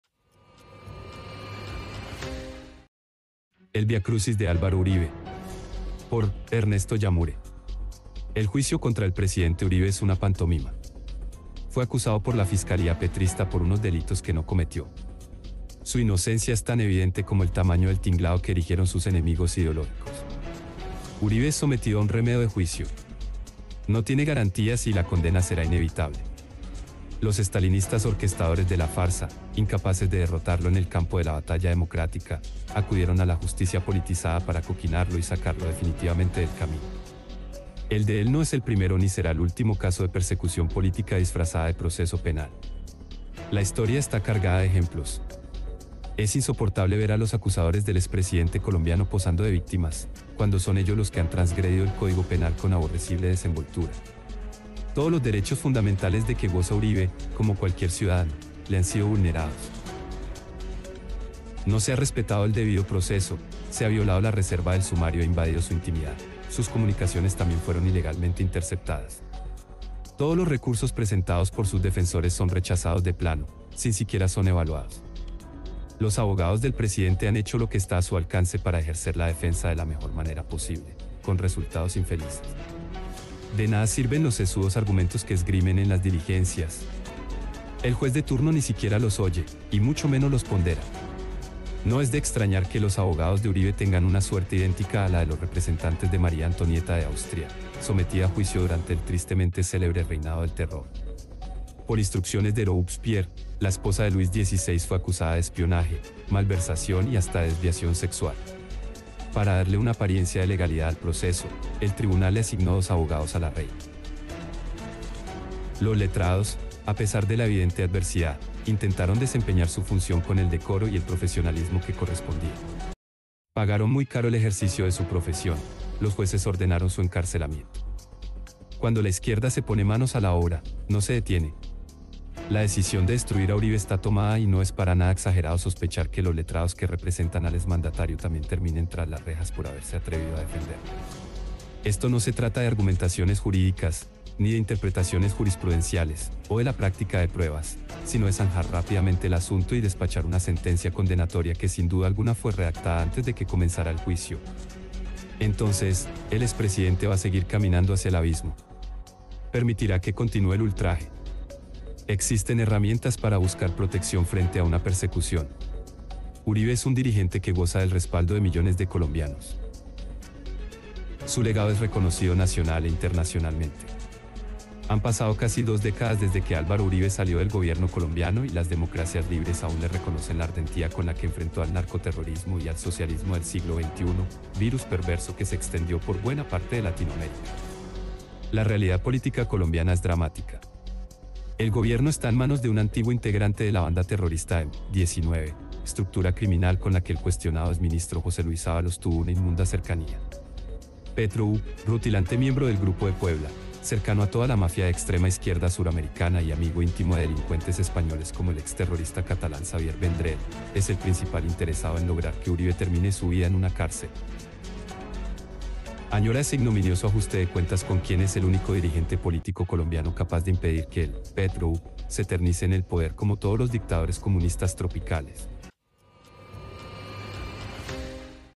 EL VIACRUSIS DE ÁLVARO URIBE || Por: Ernesto Yamhure
El juicio contra el presidente Uribe es una pantomima. Fue acusado por la Fiscalía petrista por unos delitos que no cometió. Su inocencia es tan evidente como el tamaño del tinglado que erigieron sus enemigos ideológicos.
COLUMNA ERNESTO YAMHURE